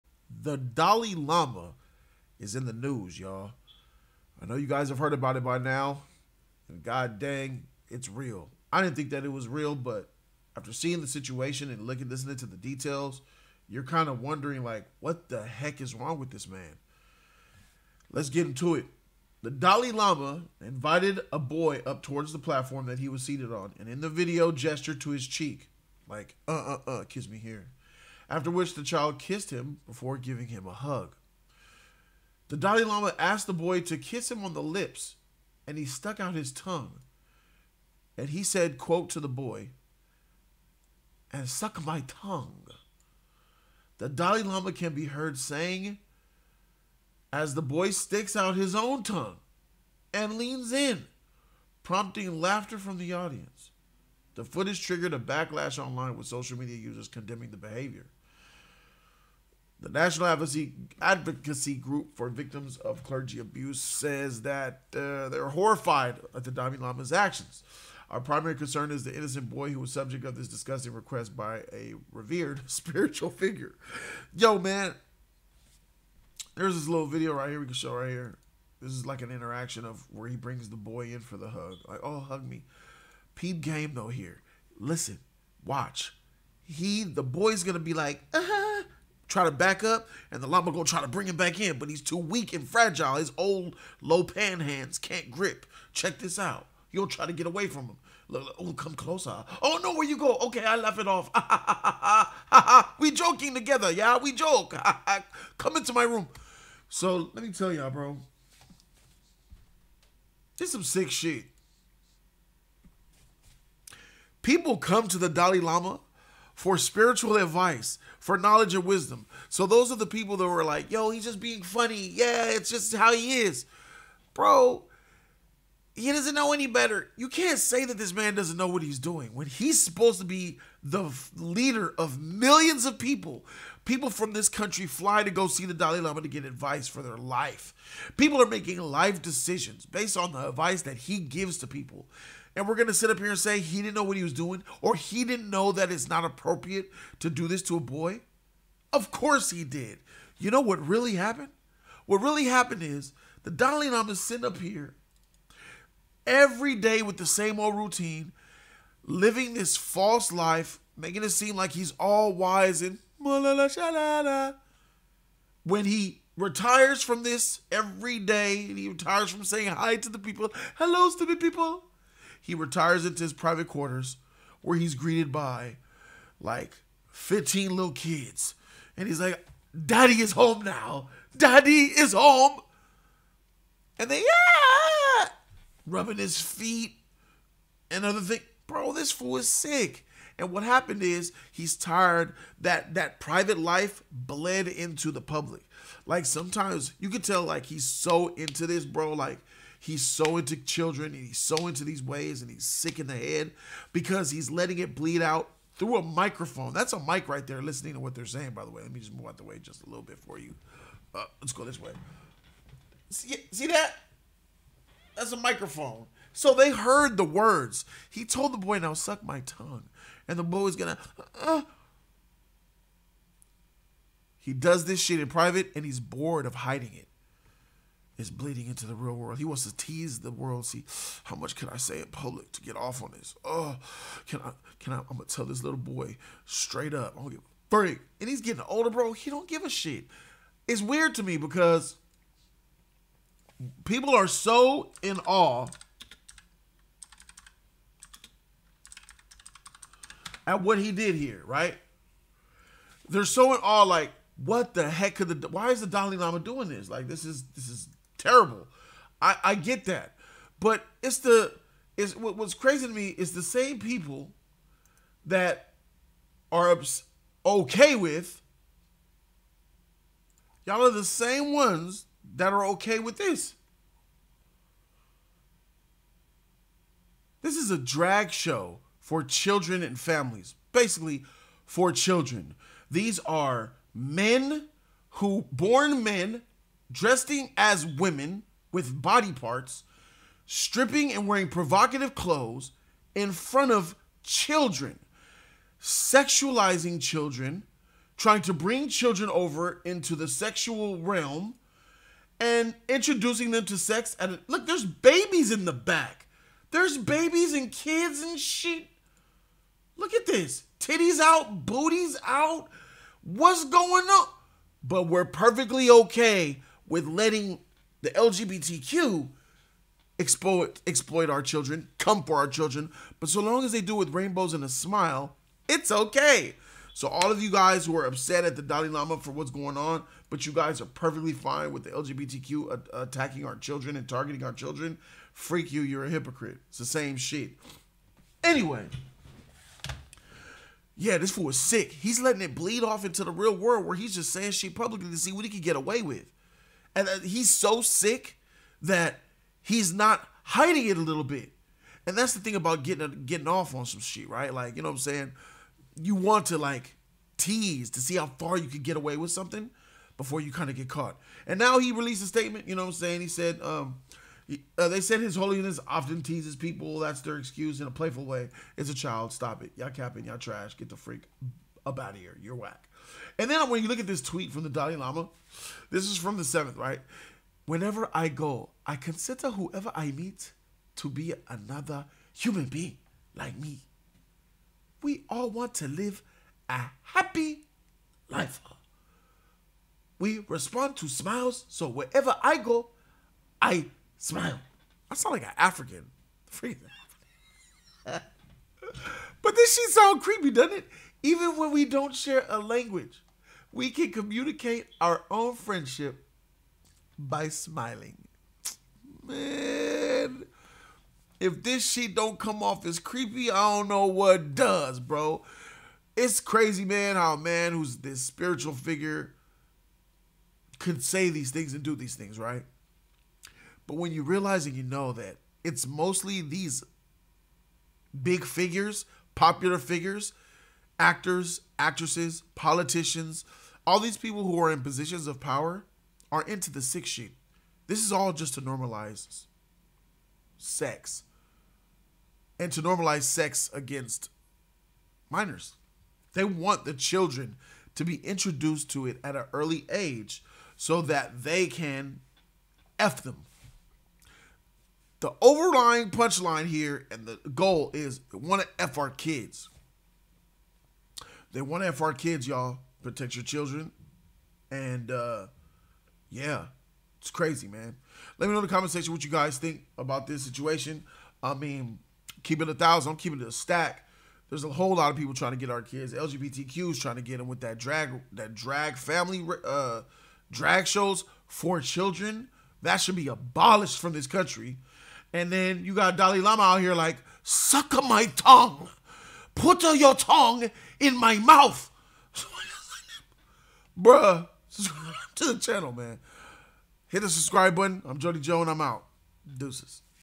The Dalai Lama...
Yo, this story is crazy! The Dalai Lama, the Tibetan spiritual leader, apologized on Monday after a video surfaced online showing him kissing a boy on the lips at a public event earlier this year and then saying to the child, “Suck my tongue.”

Podcast: Jody's Corner Live! - Spotify, Itunes, Google Music

* Add me on Facebook: jodys corner

Jody's Corner
3651 South La Brea Ave 958